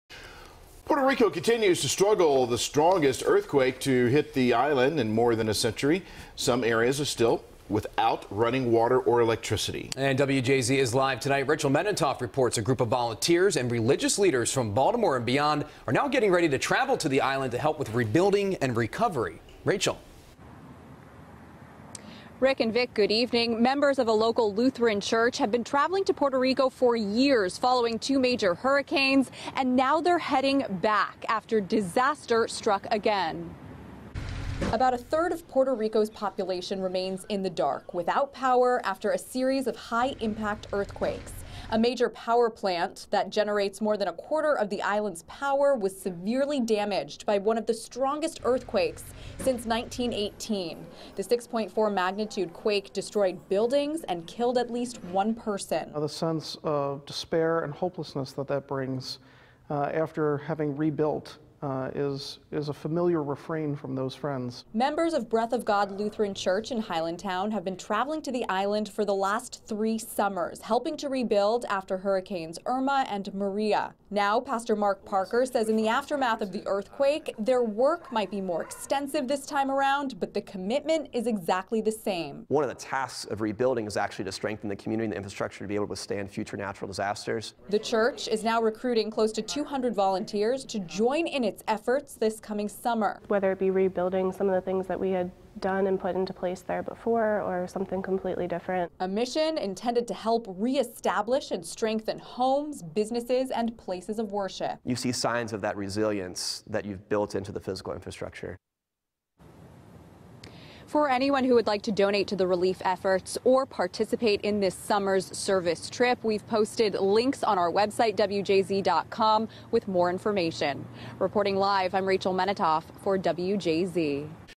Volunteers From Baltimore Plan Trip To Puerto Rico To Help With Earthquake Recovery
Puerto Rico continues to struggle after being hit by the strongest earthquake in more than a century, and as residents continue to deal with strong aftershocks and a lack of water and electricity, a group of volunteers from the Baltimore area are heading south to help aid the region's recovery.